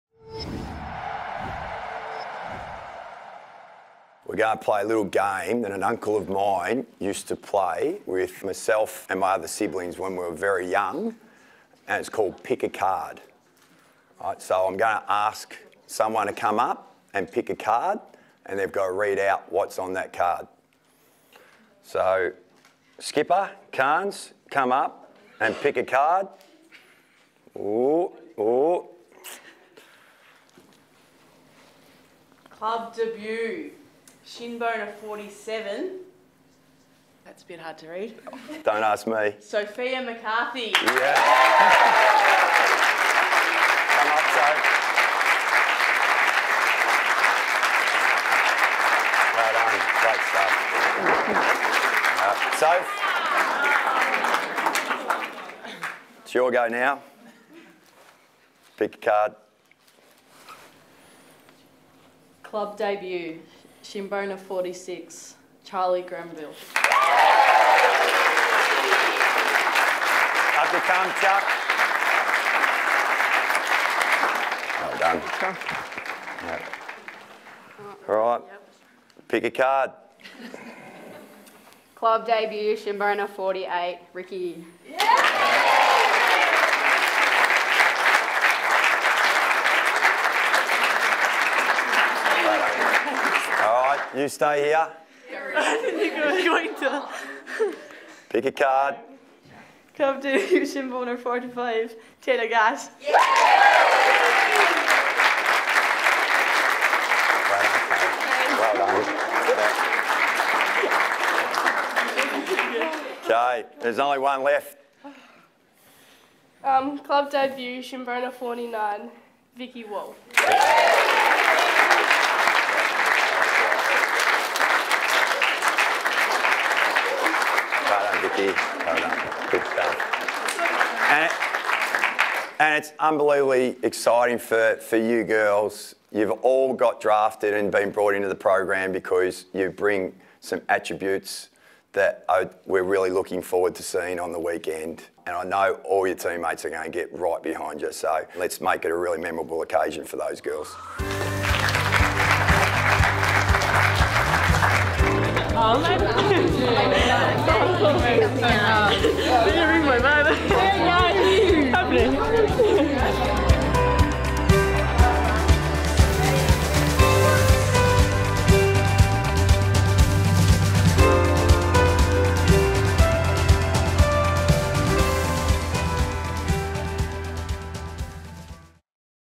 AFLW: Crock's card game with FIVE aces in the pack
AFLW senior coach Darren Crocker reveals the Kangaroos' five AFLW debutants in Taylah Gatt, Sophia McCarthy, Charli Granville, Vikki Wall and Erika O'Shea.